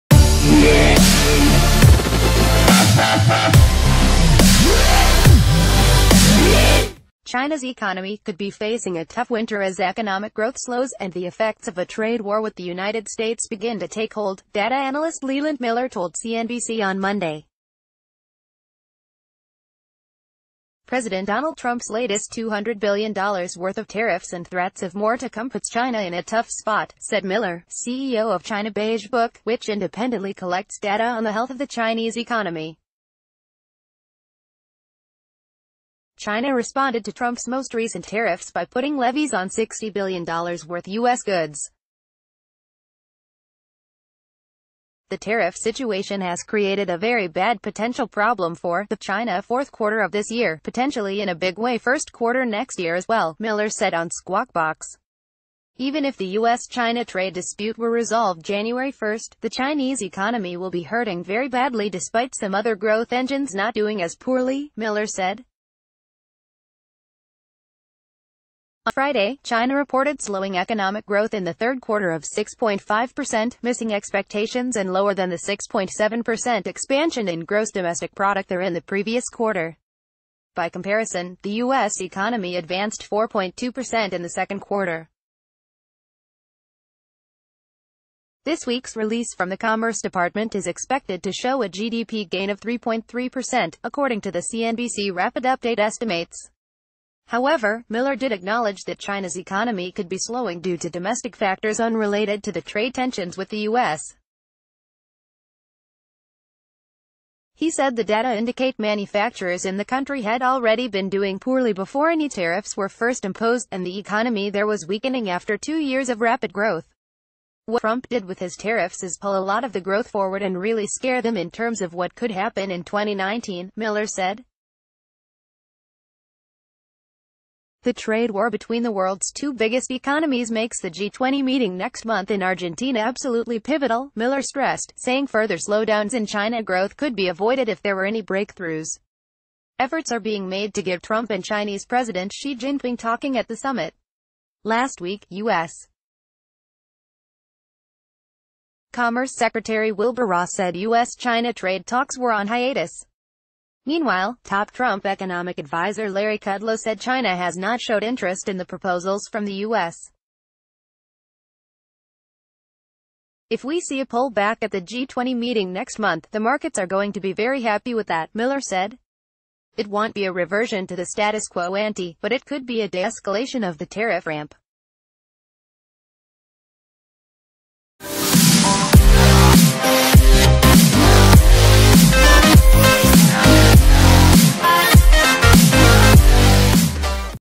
Daily News - The Trumps tariff may adversely affect China's economy over the next few months
Daily News - The Trumps tariff may adversely affect Chinas economy over the next few months

Chinas economy could be facing a tough winter as economic growth slows and the effects of a trade war with the United States begin to take hold, data analyst Leland Miller told CNBC on Monday. President Donald Trumps latest $200 billion worth of tariffs and threats of more to come puts China in a tough spot, said Miller, CEO of China Beige Book, which independently collects data on the health of the Chinese economy. China responded to Trumps most recent tariffs by putting levies on $60 billion w...